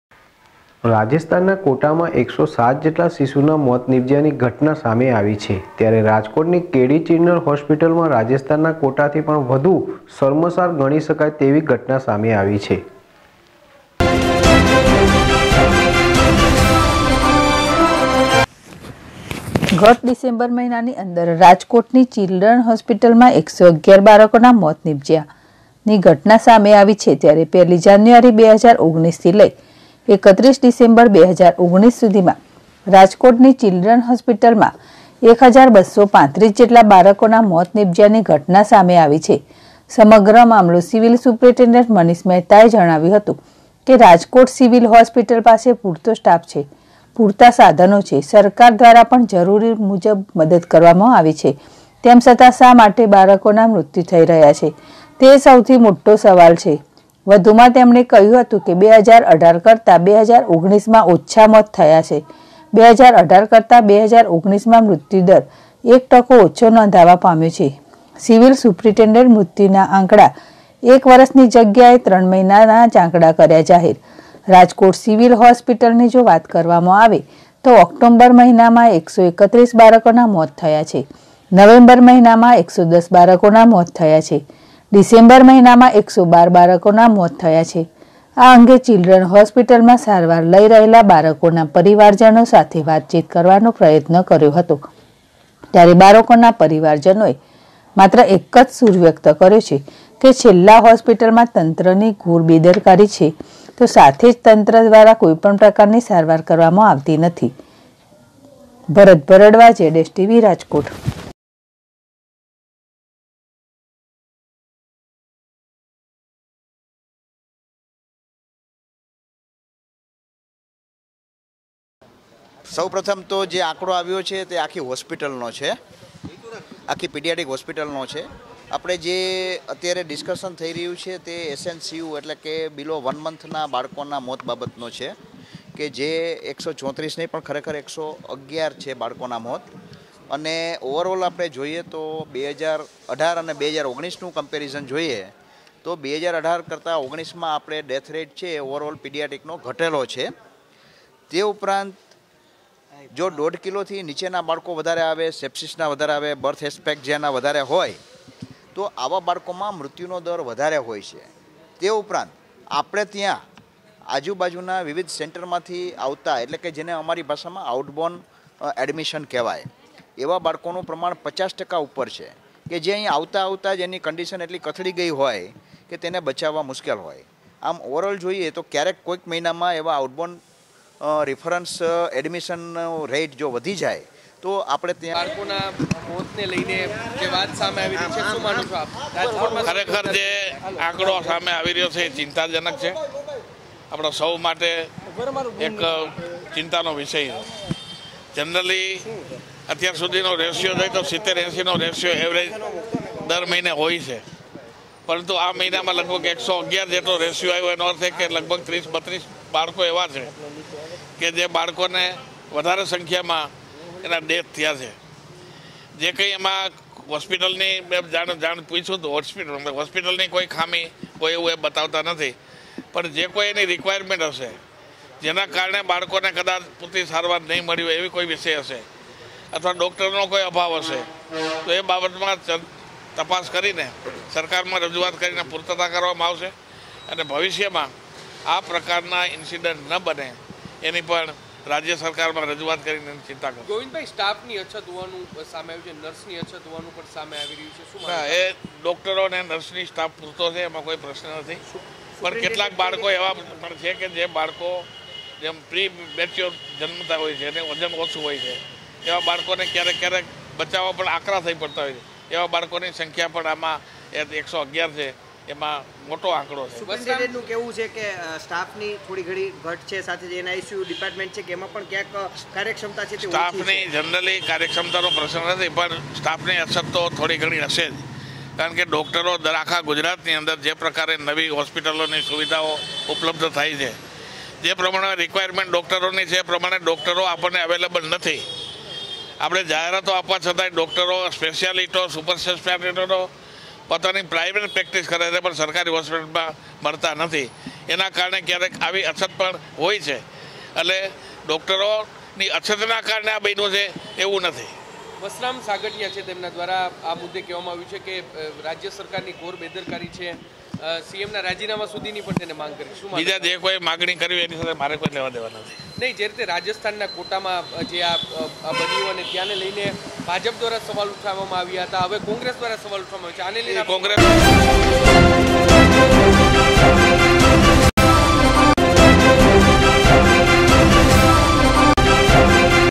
રાજયમાં નવજાત શિશુઓના મોતના આંકડાએ સરકારને કરી દોડતી //ZSTVNEWS//
રાજસ્થાનના કોટામાં ૧૦૭ જેટલા શિશુઓના મોત નિપજ્યા ની ઘટના સામે આવી છે.